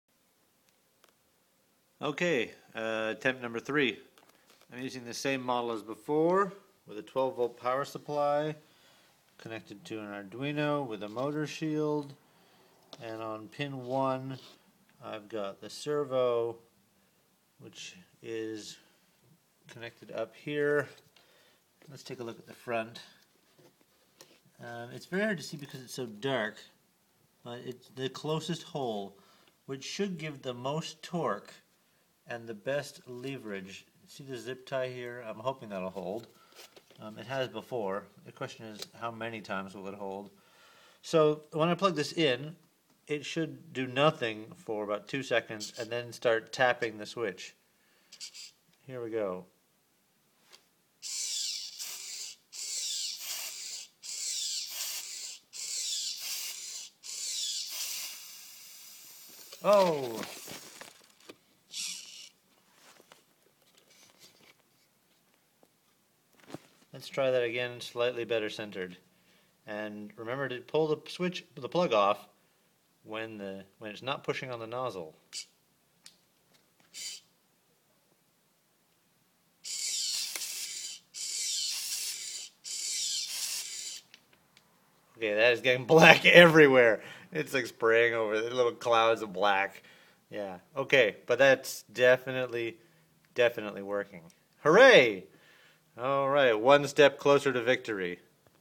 Makelangelo Venti Spray paint art robot
The Makelangelo is our internationally reknown picture drawing robot.  People asked us to go big so that's what we're working on - making a giant version to spray paint murals.  This test model will be able to cover about 4m * 4m if everything goes according to plan.

The robot uses all the same electronics as the smaller model with timing belts, sprockets, and counterweights to handle the increased weight of the pen holder.  The frame is made from birch parts designed and made in-house on our laser cutter.